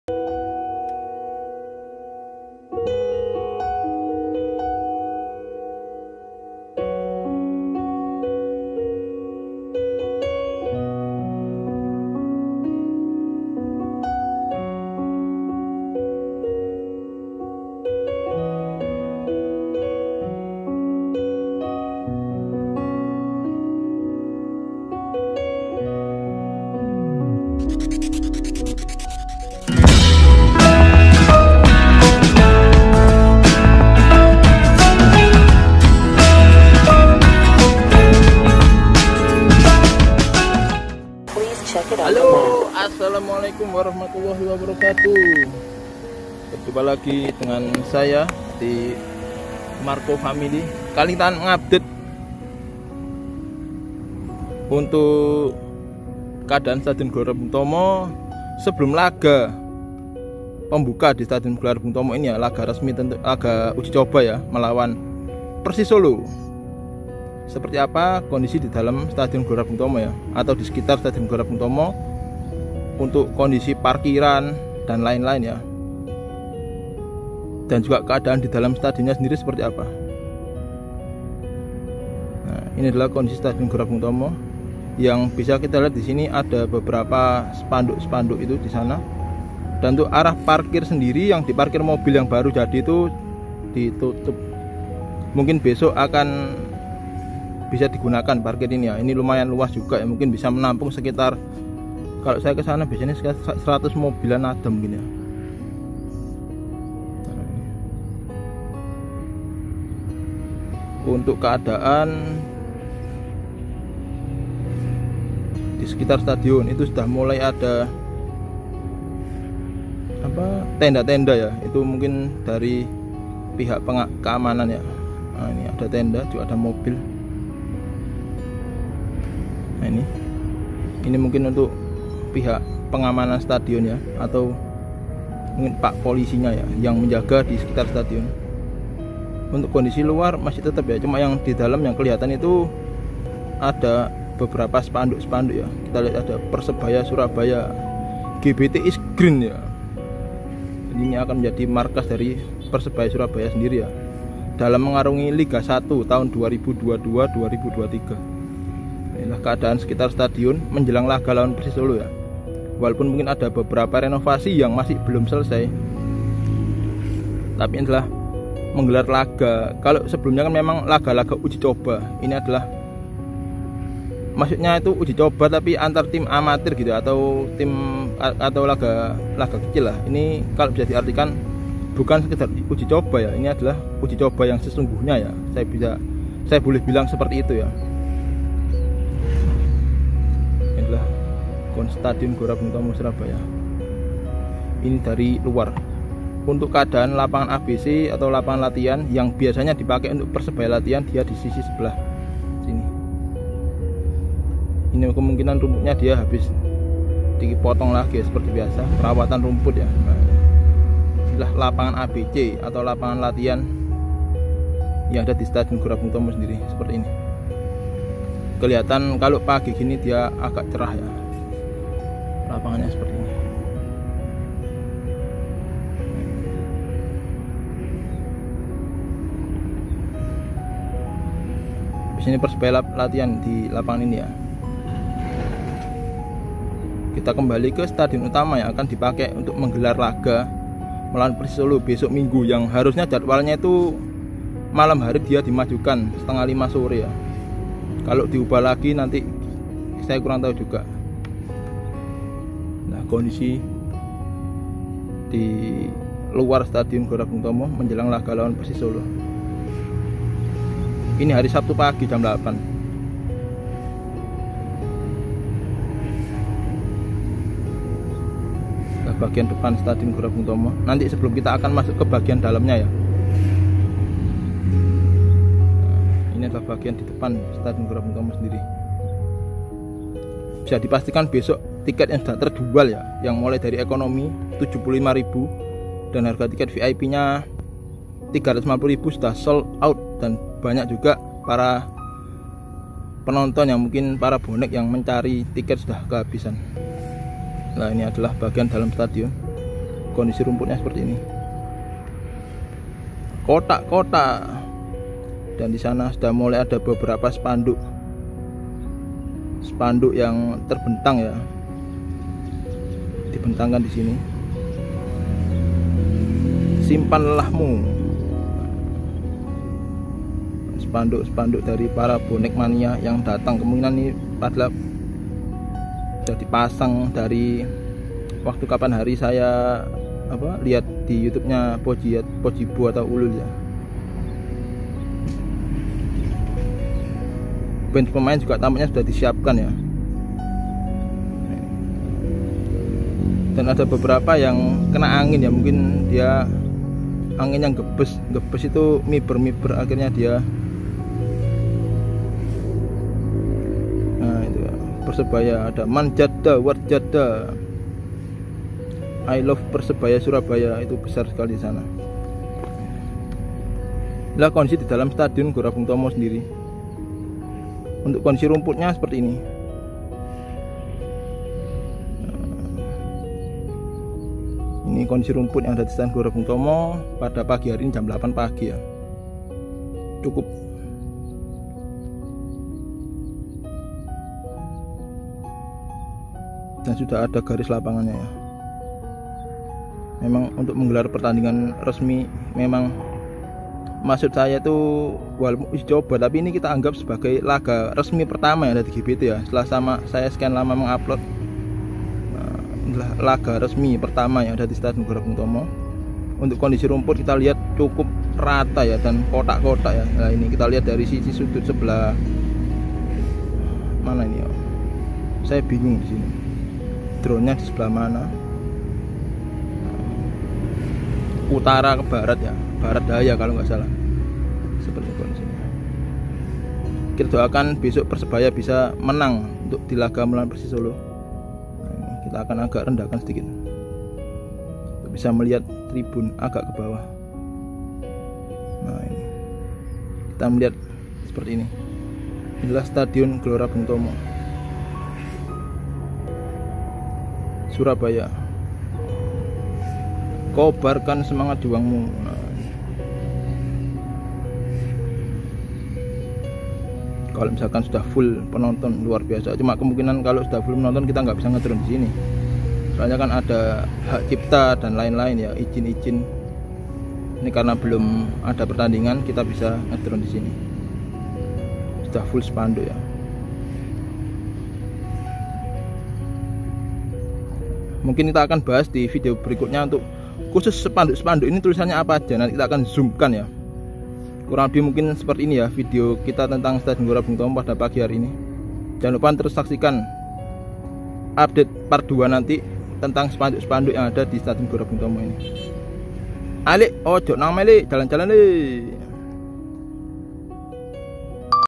PERSEBAYA DAY .!!GBT GELAR LAGA PERTAMA DENGAN SUPPORTER FULL 1 STADION - KONDISI TERKINI H-1
suasana asli stadion gelora bung tomo
sabtu pagi

sebelum laga lawan persis solo

simak videonya

video di ambil sabtu 21 mei 2022 jam 09.40

di rekam.. dji mavic air 2